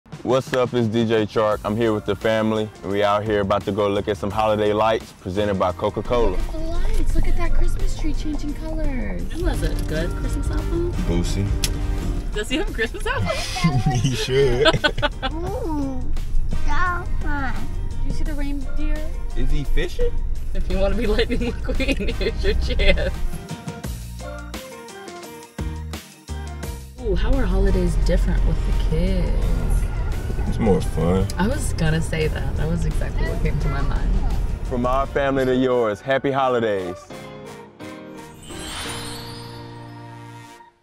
Holiday Spirit: DJ Chark and family cruise through holiday light show
Join the Chark family as they drive through a holiday light show.